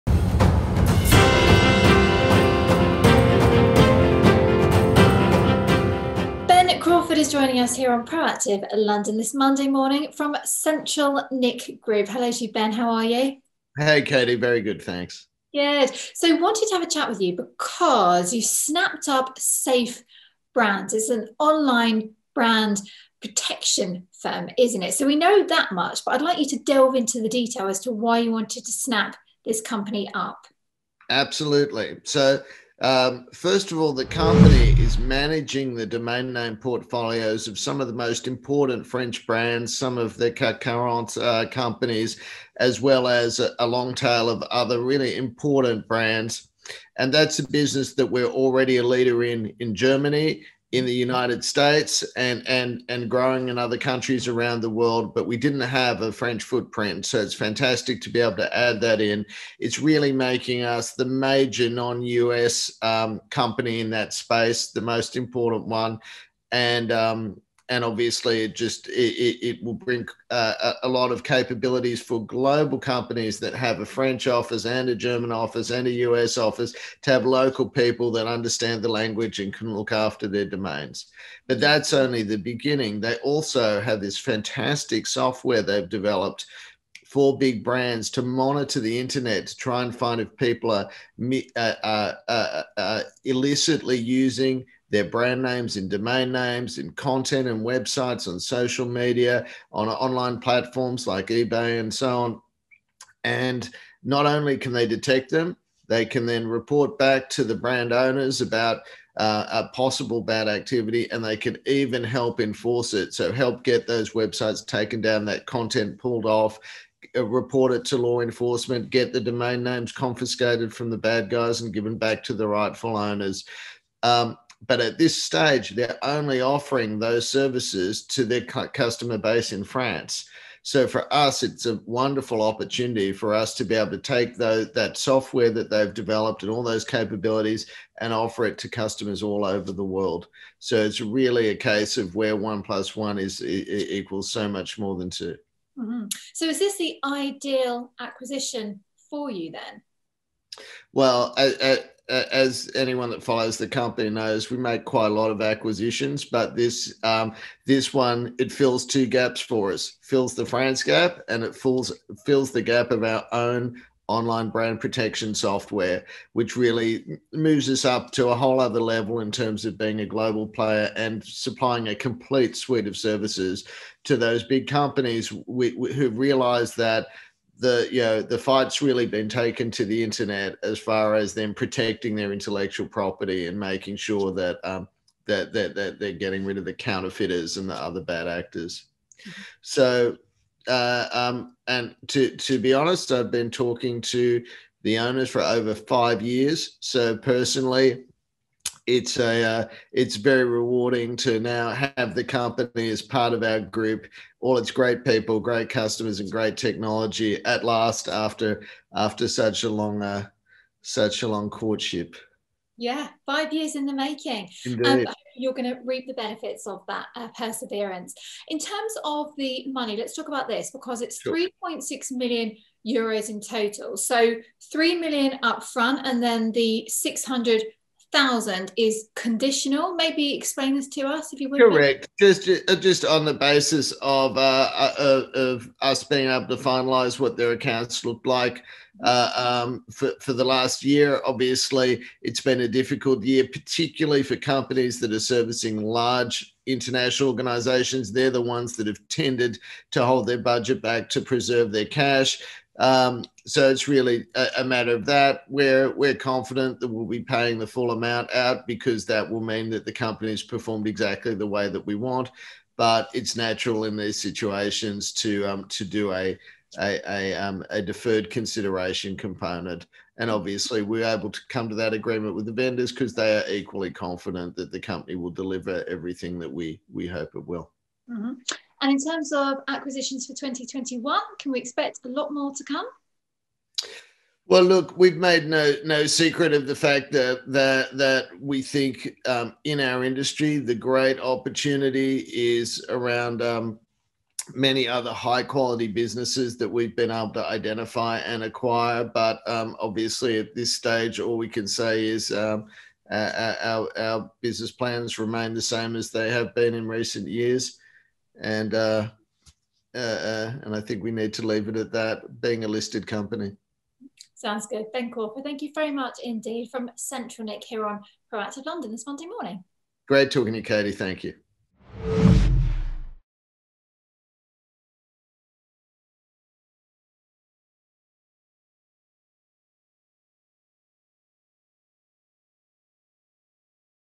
CentralNic Group snaps up 'French footprint' SafeBrands, 'making us major non-US company in space'
CentralNic Group PLC's (LON:CNIC) Ben Crawford talks to Proactive London about acquiring French firm SafeBrands.

After five years of getting to know the brand, Crawford is thrilled to now add the provider of online brand protection software and corporate internet services to their firm.

Crawford explains that CentralNic is already a leader in Germany and the United States and with France now in the mix, 'making us the major non-US company in the space'.